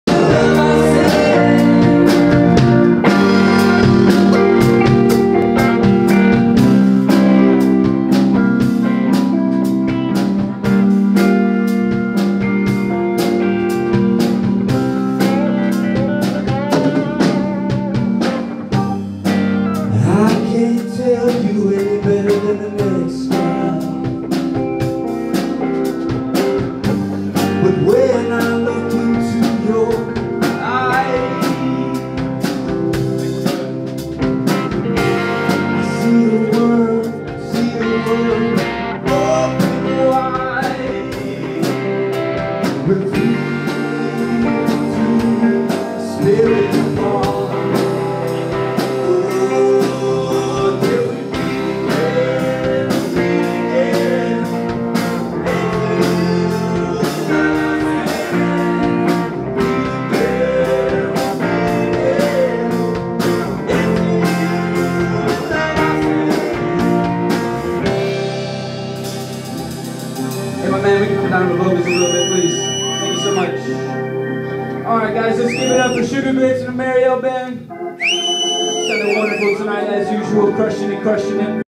Toad Head, Epicure Cafe, 10-27-18
Toad Head, Mary-El Band, and Suga Grits at Epicure Café in Fairfax VA for an awesome night of music and cool vibes!